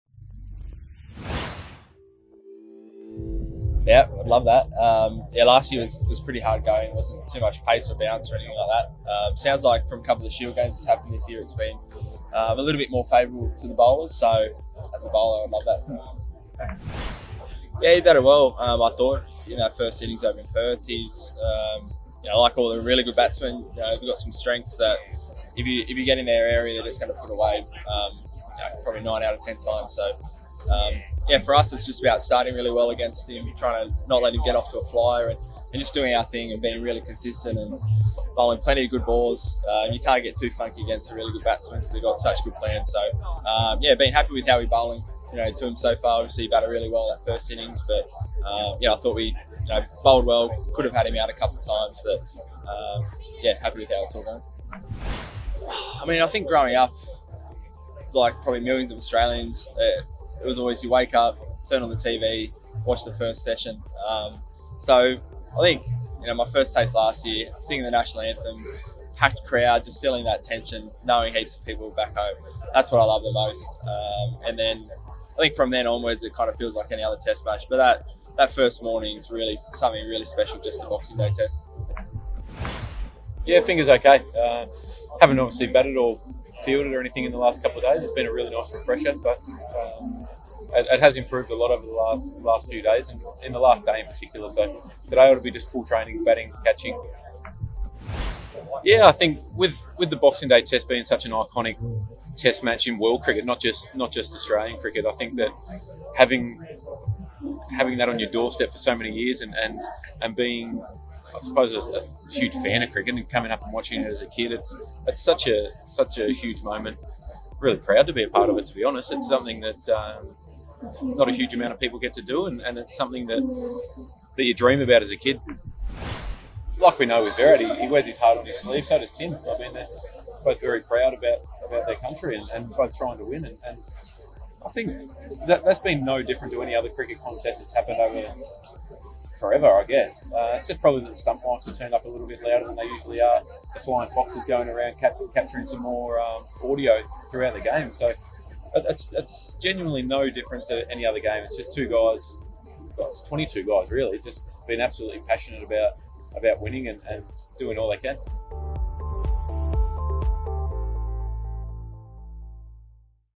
Aaron Finch and Pat Cummins speak to the media ahead of the Boxing Day Test
As the Boxing Day Test draws close, watch Aaron Finch and Pat Cummins talk to the media about the conditions, the pitch and the importance of Boxing Day Tests.